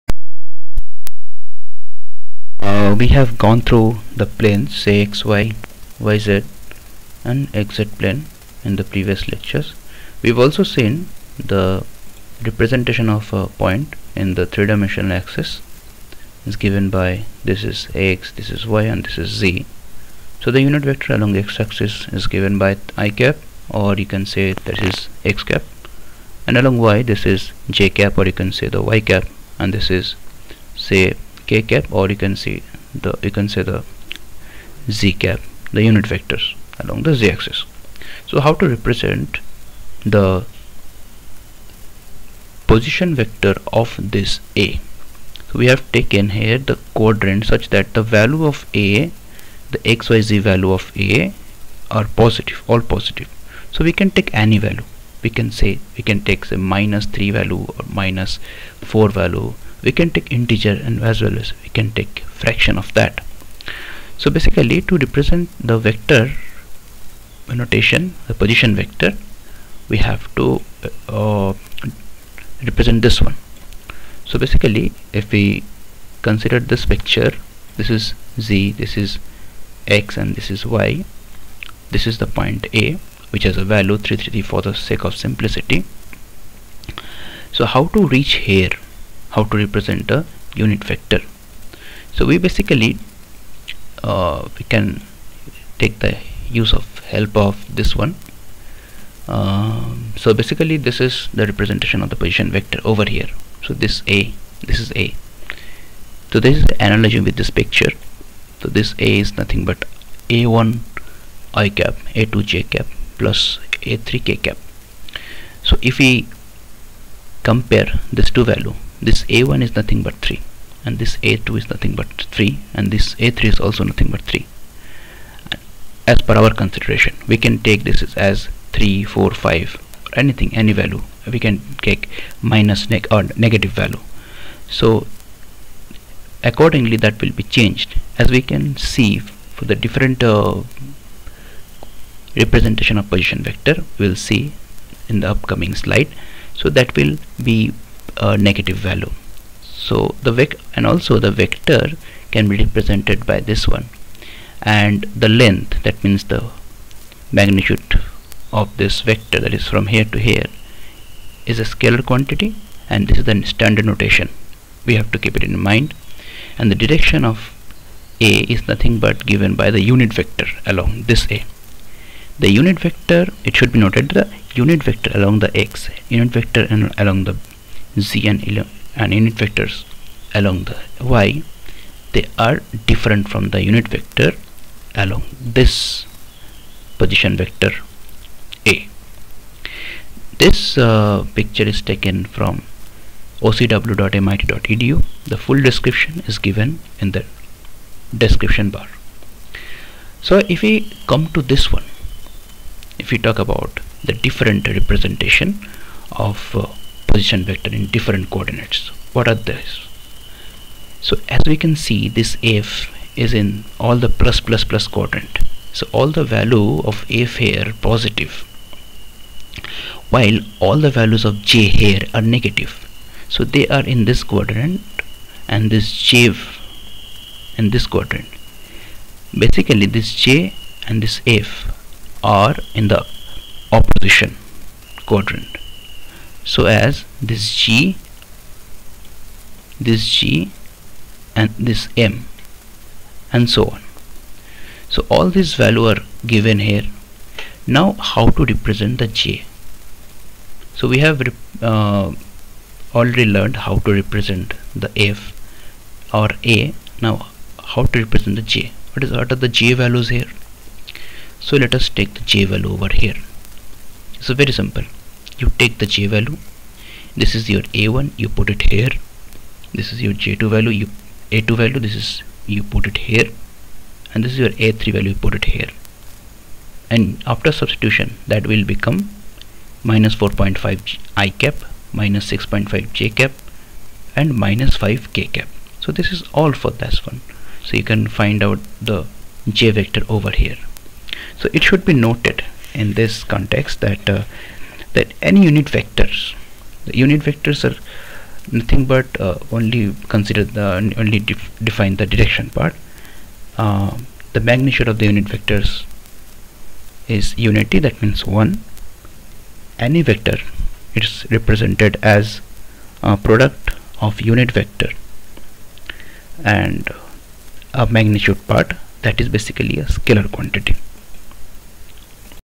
Physics and Mathematics Part IV, position vector, Class 11, CBSE, ICSE, State Boards_English
Designed for CBSE, ICSE and State Boards for the fundamental concepts of Mathematical tools for physics. Here we are introducing Vectors, one of the most important fundamental topics in Physics. We refer the books:
1) Concept of Physics by HC Verma
2) Comprehensive Physics by Narinder Kumar
3) Core Physics Class XI

4) Fundamentals of physics, extended edition [8 ed.] by David Halliday, Robert Resnick, Jearl Walker
Software: GeoGebra(OS: Linux Mint Tricia 19.3)
Fig Ref: